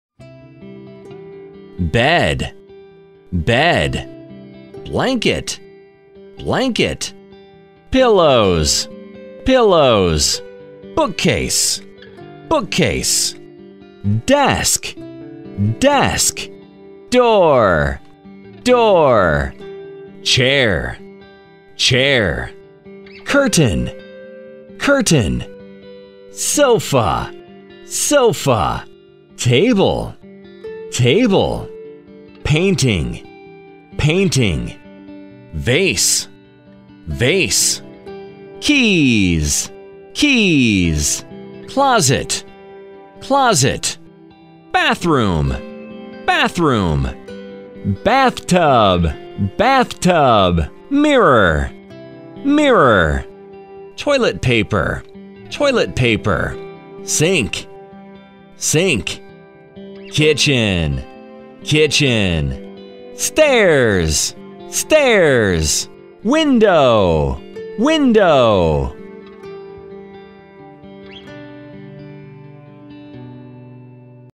English Vocabulary - Home | Home Items
We put words before the pictures, allowing audiences to focus on the recognition of the words and sounds instead of solely on the picture.

Learn English